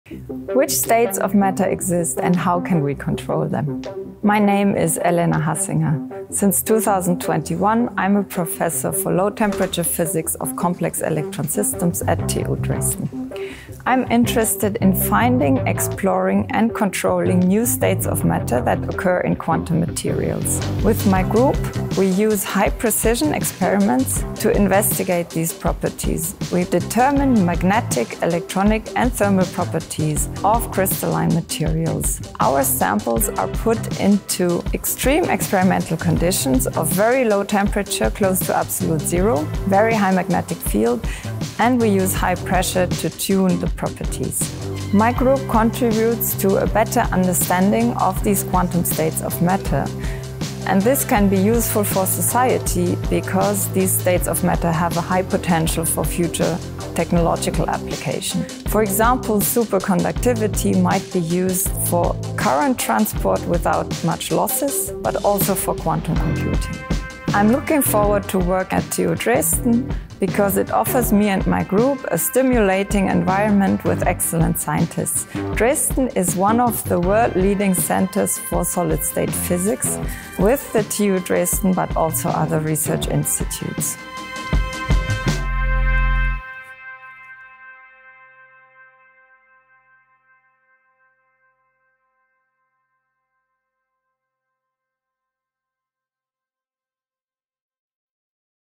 Transporting electricity without loss ⚡ | Introducing Elena Hassinger
Prof. Elena Hassinger is currently investigating extraordinary quantum phenomena that only occur when ice-age conditions are created in the lab. This is her approach to uncovering the mystery of the superconductor. Under extreme conditions such as very low temperatures, super-strong magnetic fields and high pressure, the expert in solid-state physics and her team are eliciting the quantum phenomena from new materials such as cerium rhodium arsenic (CeRh2As2). The lossless conducting of electricity using superconductors has been one of the key topics in solid-state physics for decades and has become the great hope for energy technology. The knowledge gained will help to develop superconducting materials for technical applications.

Want to learn more? On April 20, Prof. Hassinger will present her research in a university-wide inaugural lecture in the Schönfeld Lecture Hall of TU Dresden. Detailed information